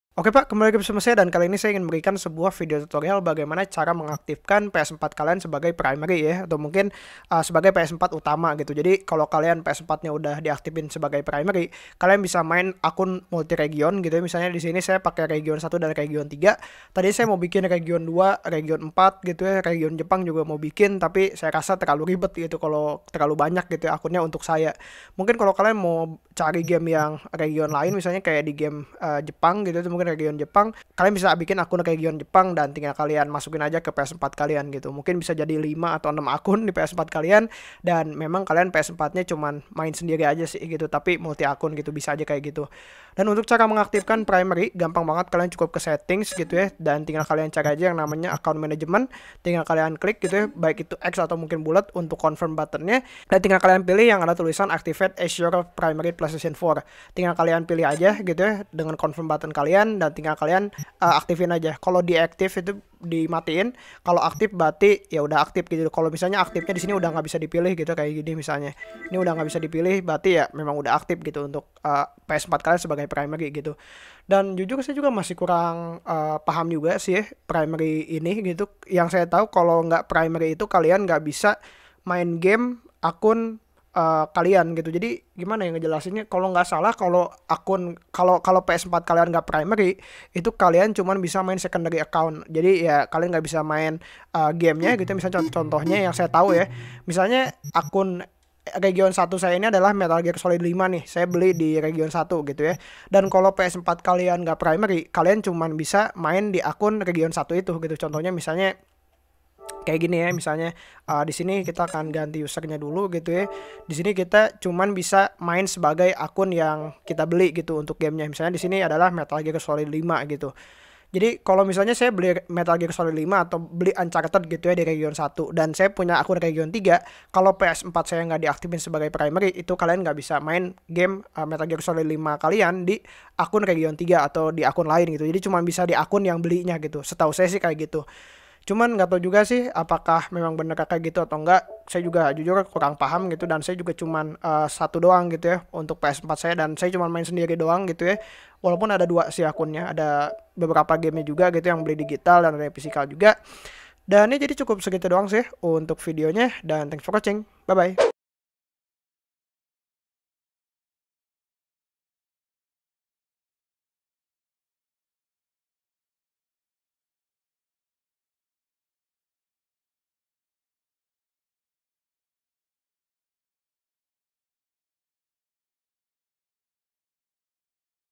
Cara Aktifkan Primary di PS4!
oke pak,kali ini guwek akan memberikan sebuah video tutorial bagaimana cara mengaktifkan ps4 sebagai primary,jadi kalian bisa main dengan banyak akun di akun utama.